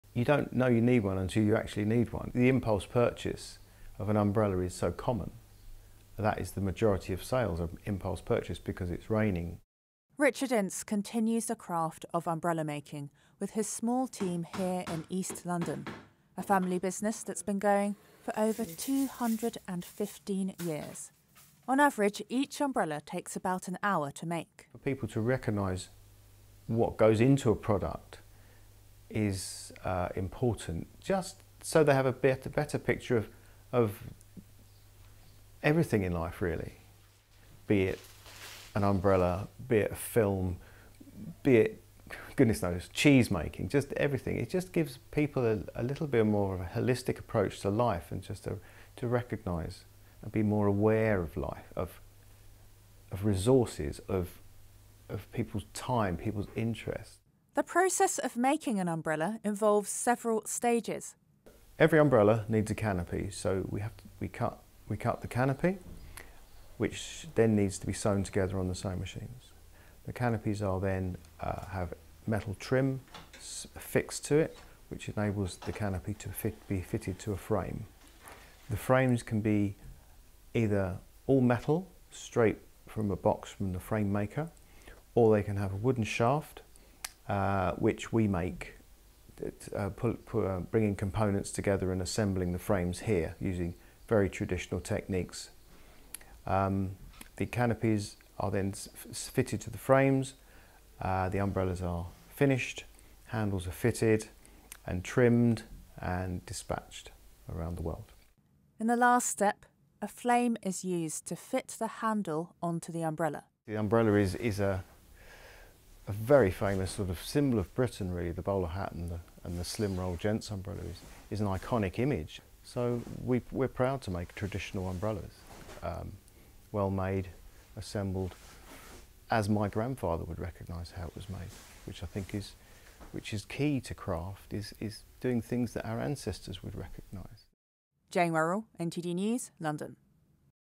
Inside the Craft of Umbrella Making
There's one place in London where you probably won't hear complaints about the rain. That's at James Ince Umbrellas, Britain's oldest umbrella maker and family business that goes back six generations. NTD met with Richard Ince to gain a glimpse into the craft of umbrella making.